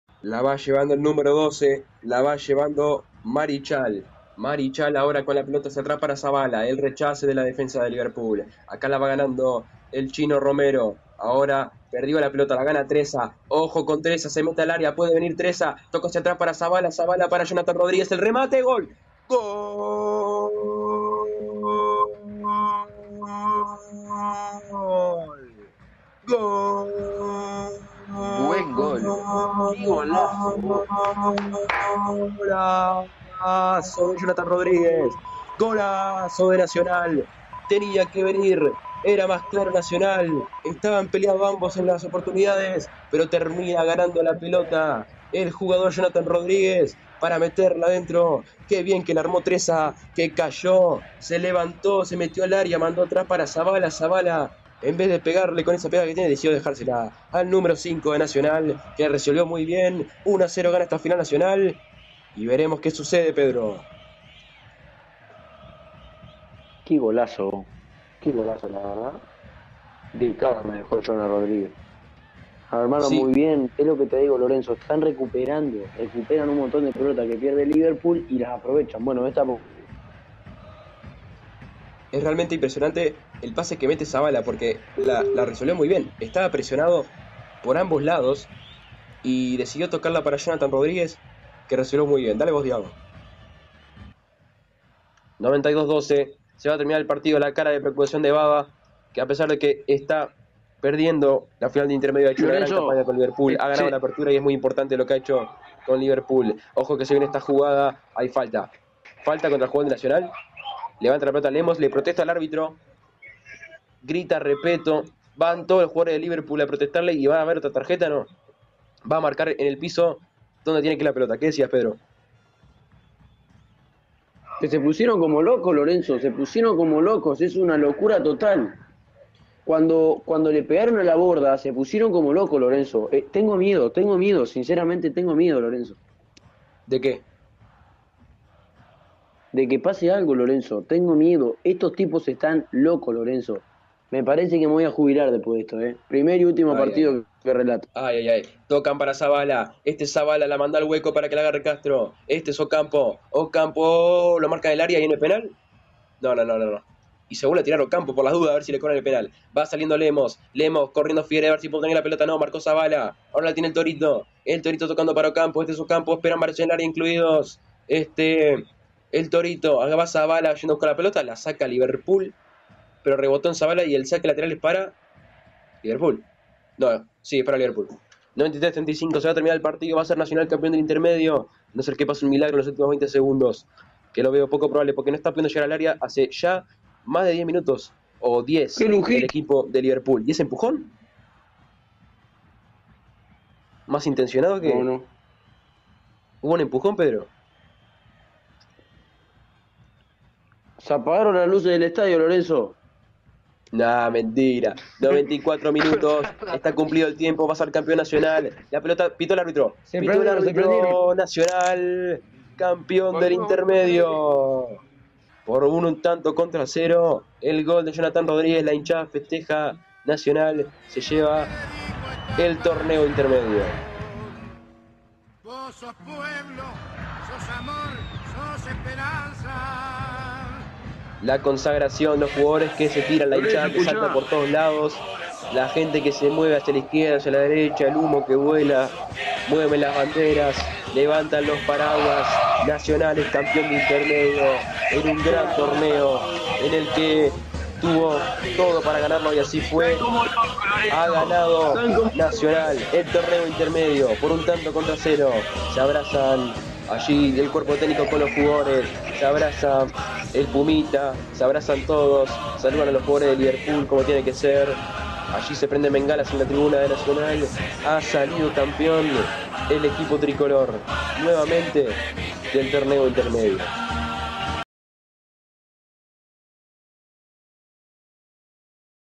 Nacional Campeón Intermedio 2022 (por Lorenzo Muníz)
El gol de Jonathan Rodríguez y el final del partido, por Lorenzo Muniz y Pedro Fernández para Φ.Radio.